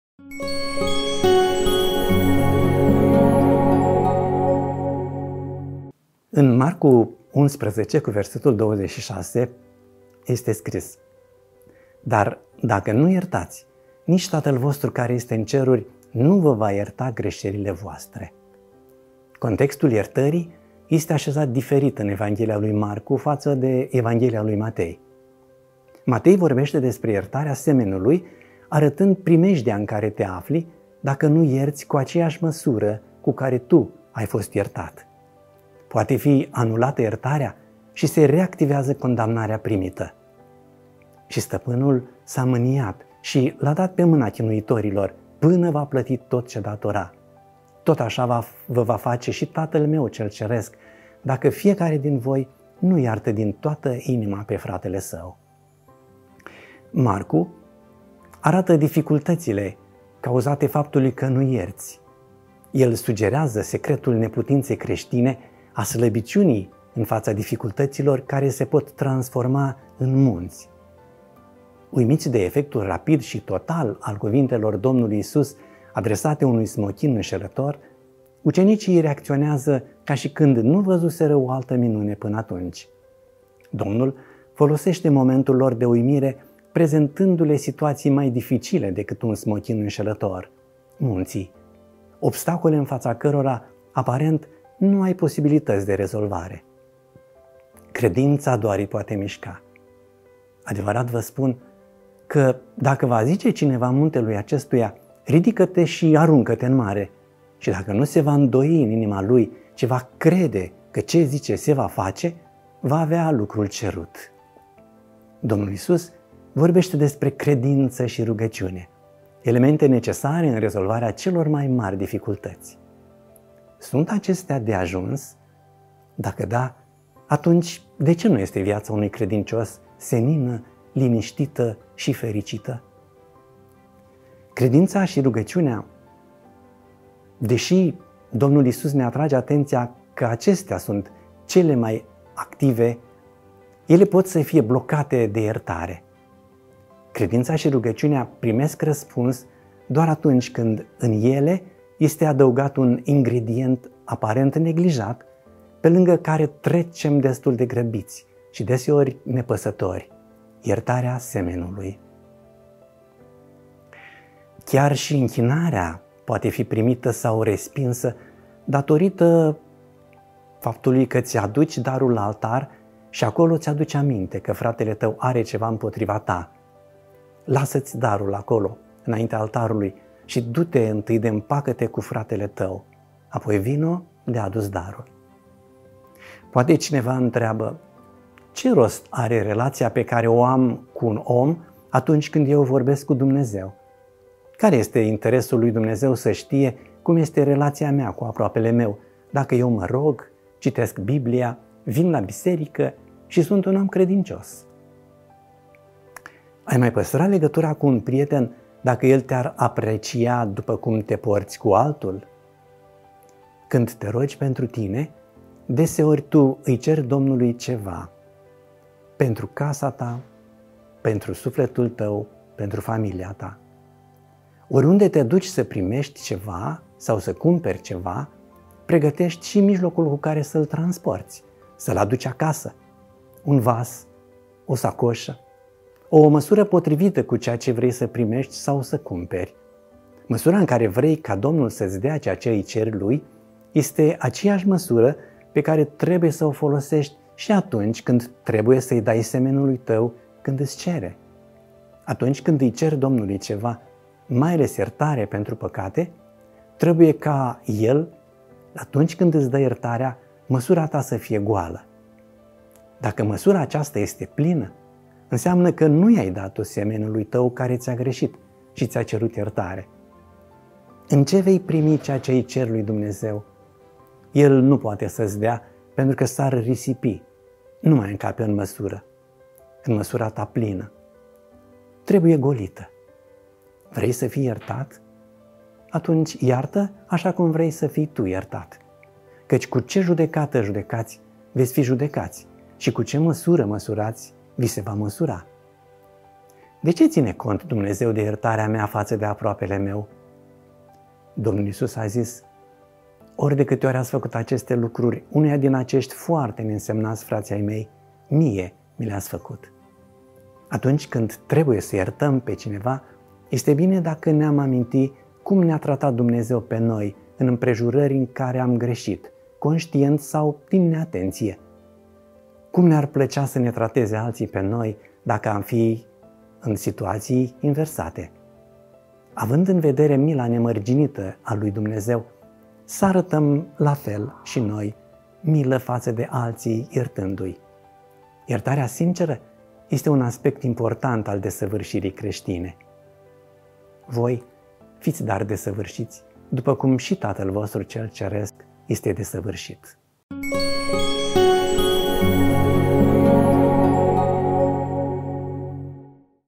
Promisiune Zilei, 27 noiembrie, cu Vadic Tanase
Dumnezeu are o promisiune minunată pentru tine în fiecare zi - primește-o prin mesajul transmis de pastorii Bisericii Adventiste de Ziua a Șaptea din Muntenia.